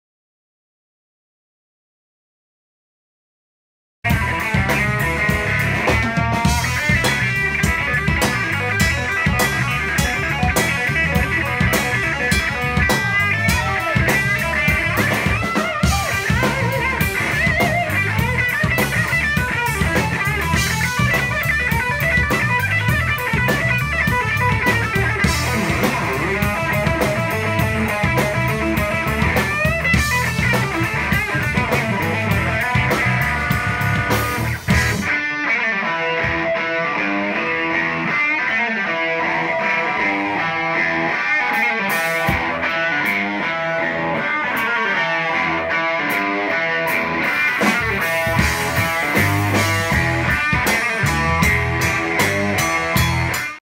Christone "Kingfish" Ingram, Oxford, MS on Oct 23, 2015.
Just a short clip of a very young Christone "Kingfish" Ingram playing at Rooster's Blues House in Oxford, MS on Oct 23, 2015.